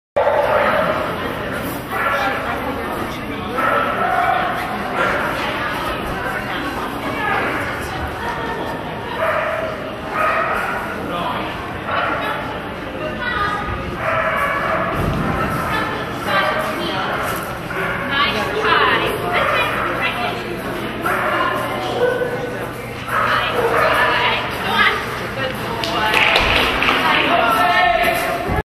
Foggy Novice Chances #1 11/27/2009
NADAC trial hosted by ZAP. NQ. Did lovely on the "chances" (distance) part, but didn't quite perform the weaves correctly.